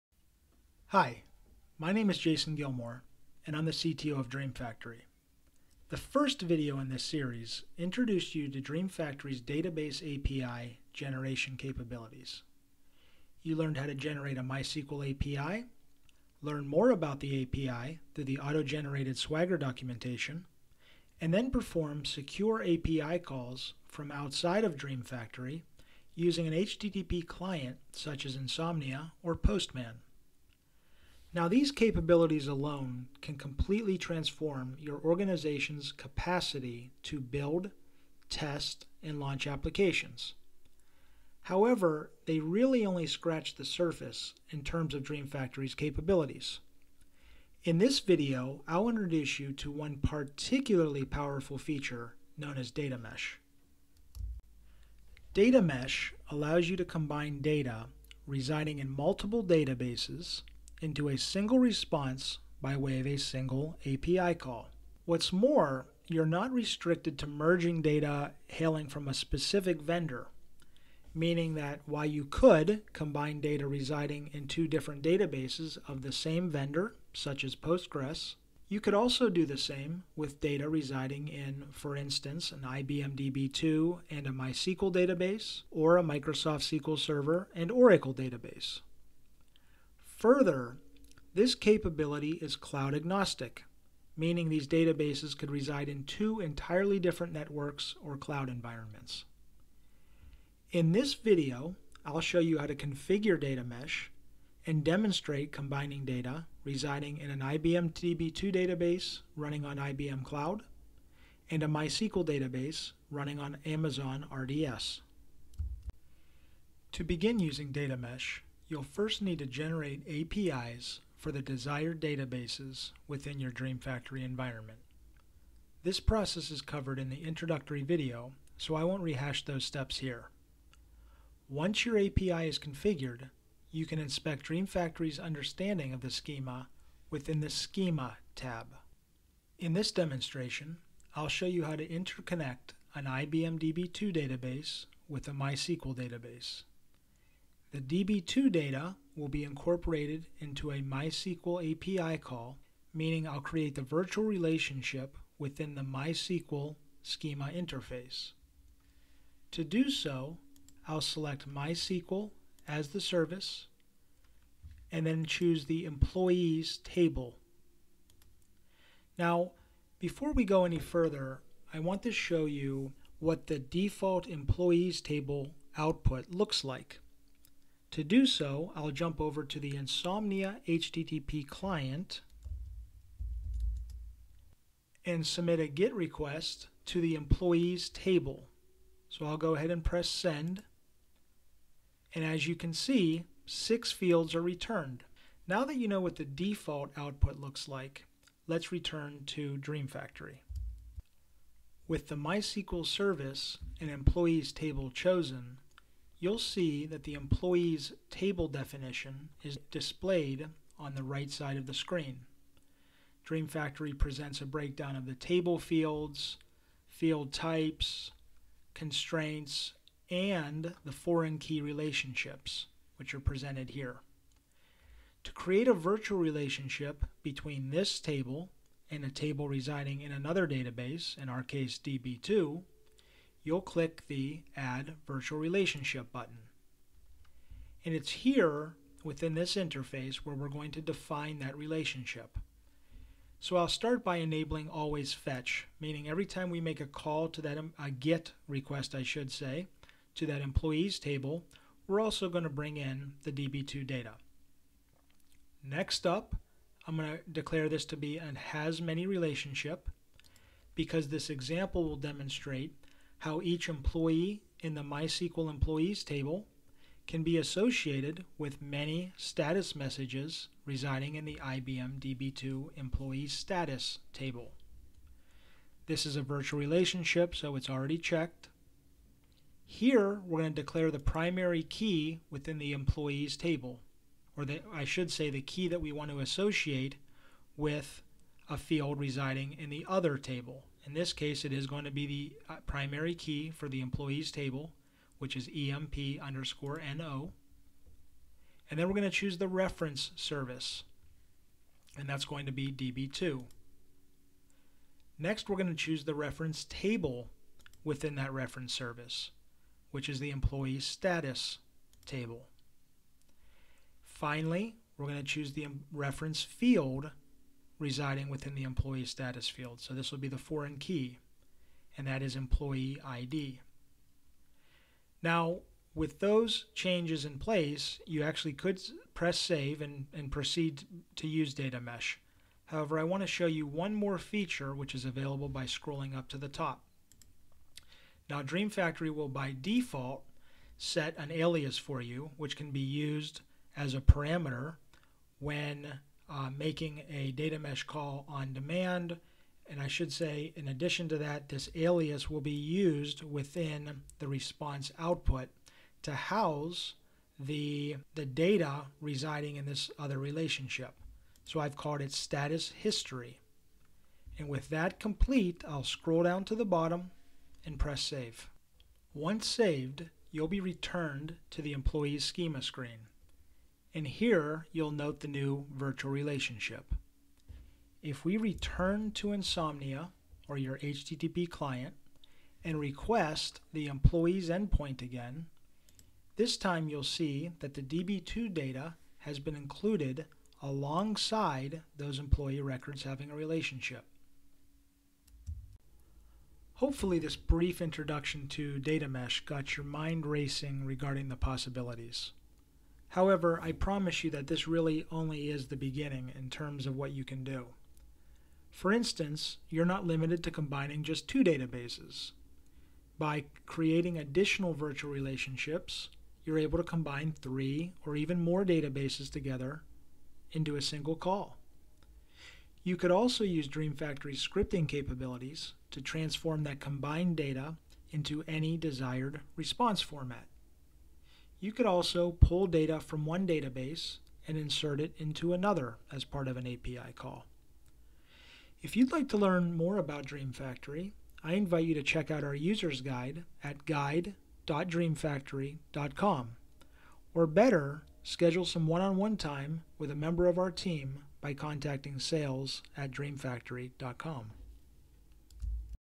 Merging Database API Calls with DreamFactory Data Mesh
Enterprises often manage data in multiple databases, often hailing from different vendors and even residing in different clouds. Thanks to DreamFactory's data mesh feature, it's possible to configure your REST API to merge data from disparate databases and present this data within a single response.